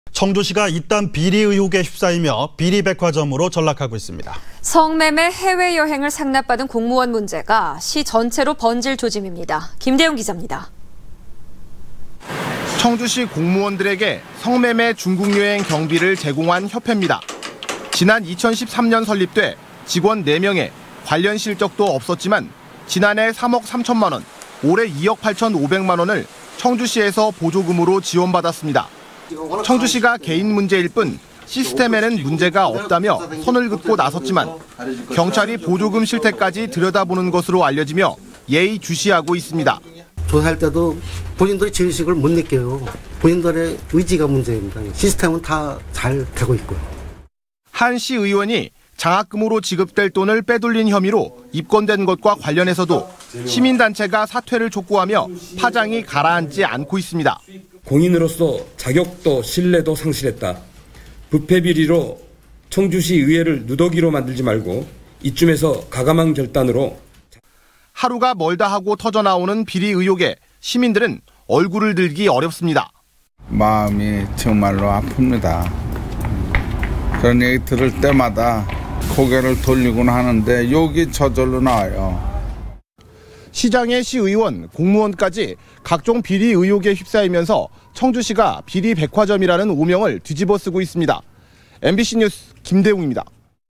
[청주MBC뉴스] 청주시 '비리 백화점' 오명
◀앵커▶
 청주시가 비리 백화점이란 오명에 시민들이
얼굴을 들 수가 없습니다. 성매매 해외여행을  상납받은 공무원 문제가 시 전체로 번질
조짐입니다. 김대웅 기자입니다.
           ◀ＥＮＤ▶

           ◀ＶＣＲ▶
 청주시 공무원들에게
성매매 중국 여행 경비를 제공한 협회입니다.

 지난 2013년 설립돼
직원 4명에 관련 실적도 없었지만,
지난해 3억 3천만 원, 올해 2억 8천5백만 원을
청주시에서 보조금으로 지원받았습니다.

 청주시가 개인 문제일 뿐 시스템에는
문제가 없다며 선을 긋고 나섰지만
경찰이 보조금 실태까지 들여다보는 것으로
알려지며 예의주시하고 있습니다.

           ◀ＳＹＮ▶
김은용/청주시 감사관
"제도가 아무리 좋아도 의식이 문제"

 한 시의원이 장학금을 지급될 돈을
빼돌린 혐의로 입건된 것과 관련해서도
시민단체가 사퇴를 촉구하며
파장이 가라앉지 않고 있습니다.

           ◀ＳＹＮ▶
홍성학/충북참여연대 사회인권위원장
"공인으로서 자격 없어, 스스로 사퇴해야"

 하루가 멀다하고 터져 나오는 비리 의혹에
시민들은 얼굴을 들기 어렵습니다.

           ◀ＩＮＴ▶
오세찬/청주시 모충동
"그런 소식 들을 때마다 정말 욕이 나온다"

(S/U) 시장에 시의원, 공무원까지
각종 비리 의혹에 휩싸이면서
청주시가 비리 백화점이라는
오명을 뒤집어 쓰고 있습니다.